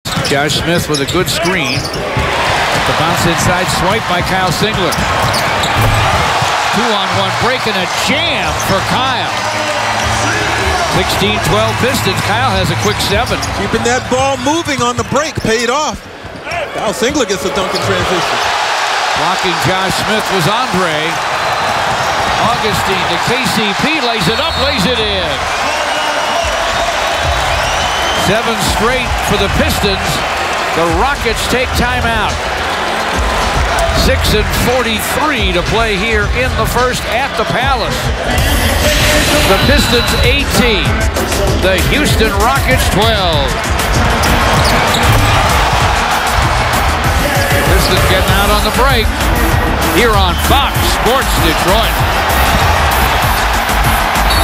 Singler & Drummond welcome Josh Smith back to Detroit
Houston Rockets vs. Detroit Pistons (January 31, 2015). The Pistons get off to a hot start in their first game against Josh Smith since waiving him. Kyle Singler finishes with a dunk past Smith, while Andre Drummond blocks him on the other end.